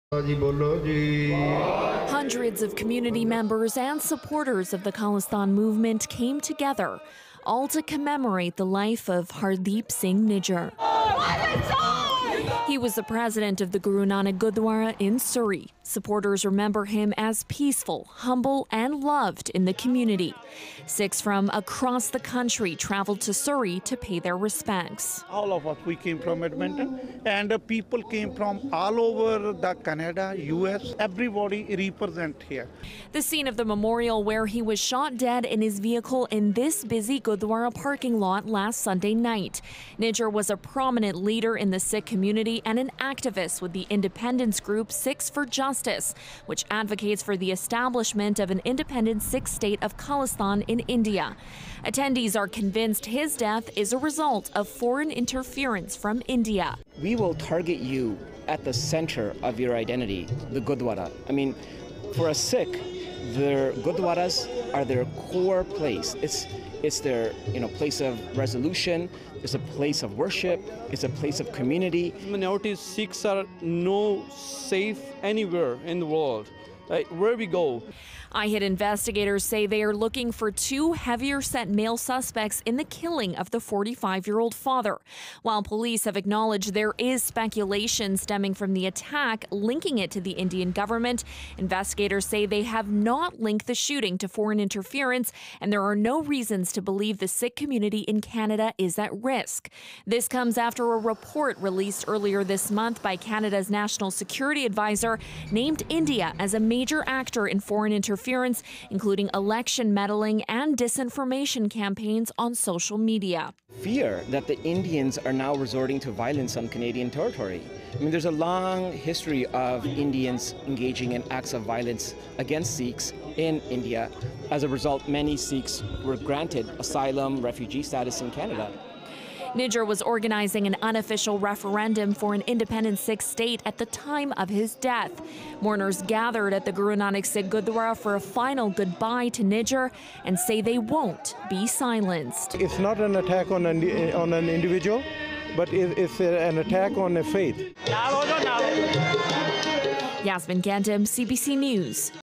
Thousands attended the funeral of slain Sikh leader Hardeep Singh Nijjar
The funeral for slain Sikh leader Hardeep Singh Nijjar was Sunday at the Surrey Gurdwara, where he was shot and killed on Father's Day.  As Yasmin Gandham reports, his death has sparked fear and outrage.